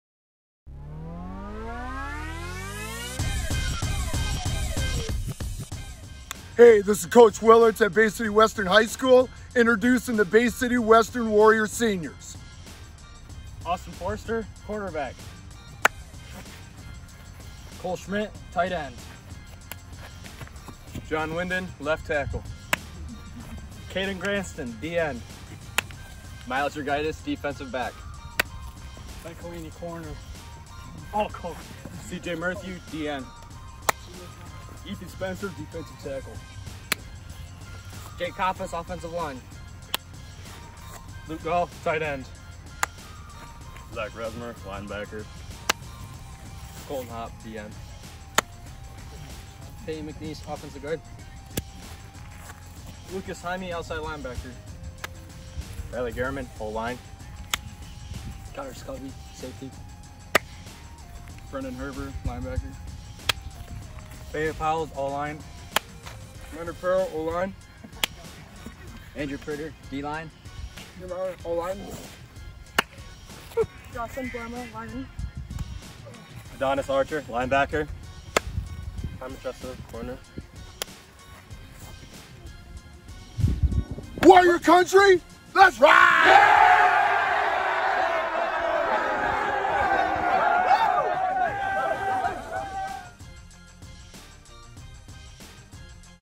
Meet the 2022 Bay City Western football team
Coach Chris Willertz prepares the Warriors for the high school football season, by Lee Thompson